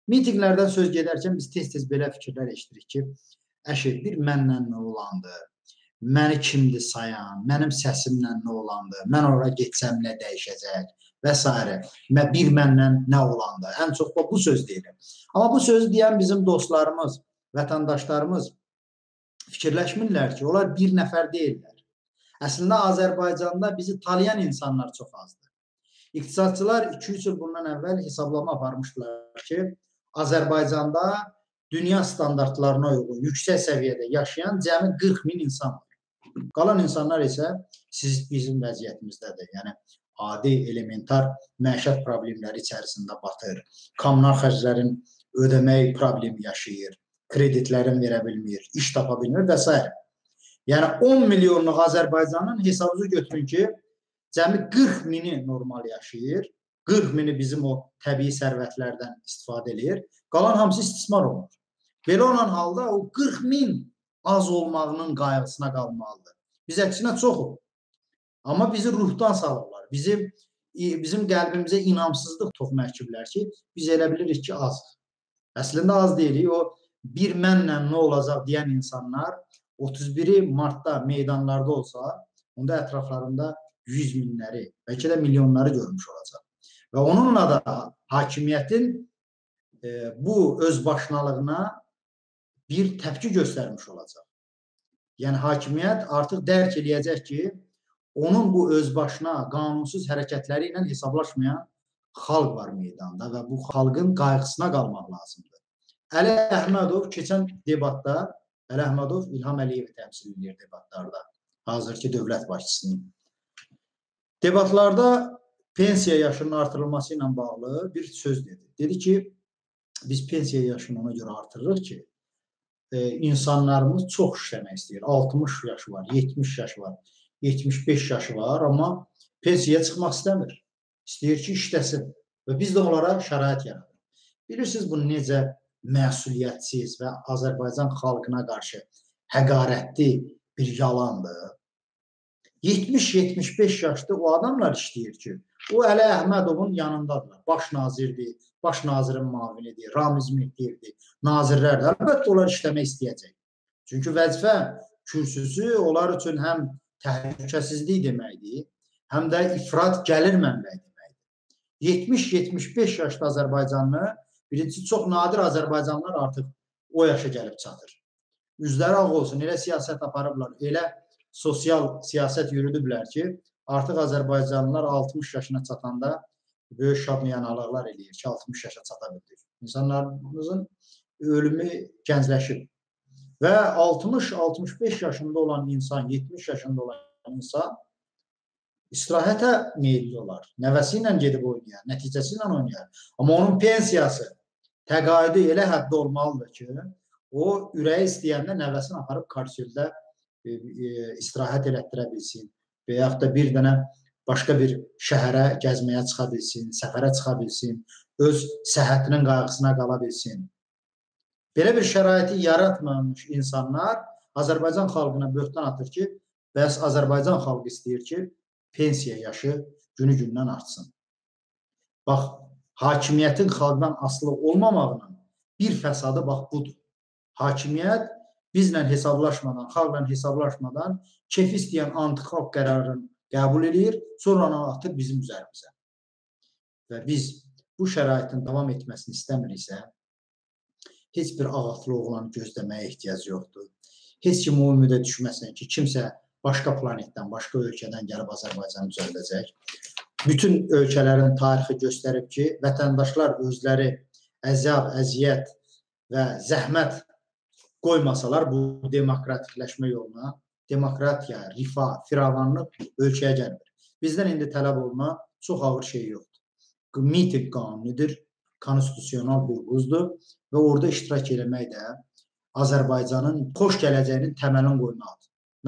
Mitinqə bir milyon insan gələ bilərmi?
31 martda Bakıda, "Məhsul" meydanında mitinq keçiriləcək.
Ölkəmiz işsizlik, haqsızlıq, özbaşınalıqdan əziyyət çəkir. Bu problem milyonlarla insanı narahat edir. Amma o insanlar elə bilirlər ki "TƏK"dilər. Ona görə də ümidsizliyə qapılıb, "əşi bir mənlə nə olandı" deyirlər. Halbuki o təklər meydana çıxsa biz milyonları görəcəyik.

Digər internet ünvanlarım

Videonu bəyənib, abunə olsanız və paylaşsanız sizdən yerdən göyəcən razı qalaram